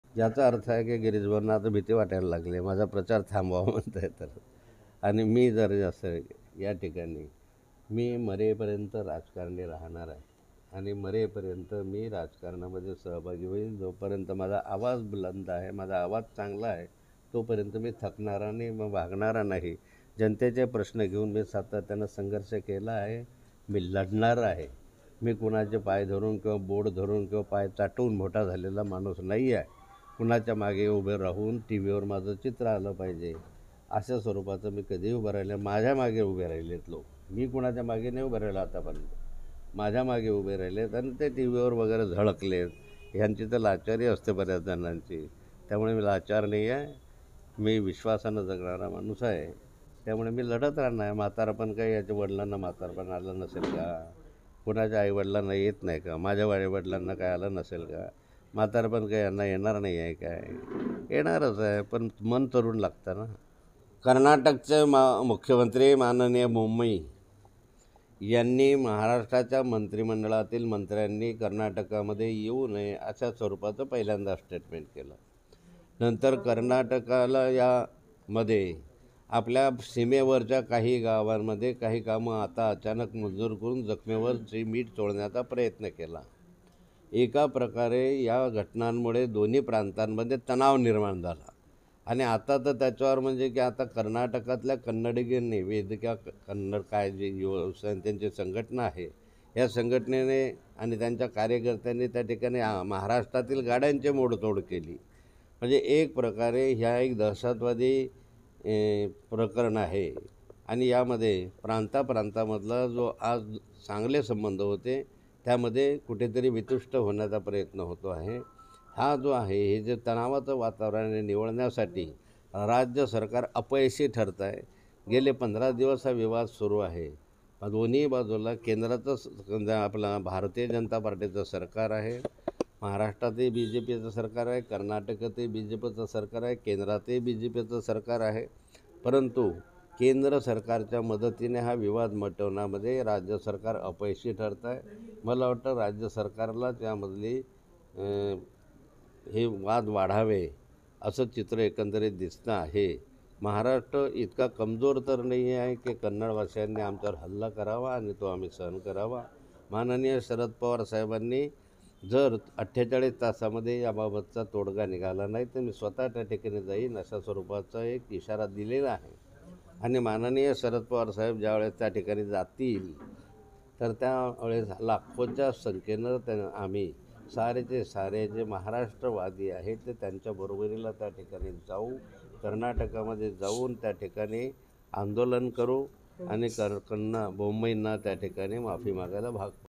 Girish Mahajan on Eknath Khadse | खडसेंचं महाजनांना प्रत्युत्तर | Maharashtra Times
एकनाथ खडसेंनी आराम करावा जास्त फिरू नये असे म्हणत गिरीश महाजनांनी टोला लगावला. यावर एकनाथ खडसेंनी जोरदार प्रतिउत्तर दिले. माझा आवाज बुलंद,मी मरेपर्यंत राजकारण करेन;असं खडसे म्हणाले. मी कुणाचा पाय चाटून मोठा झालो नाही, असं म्हणत खडसेंनी महाजनांना सुनावलं.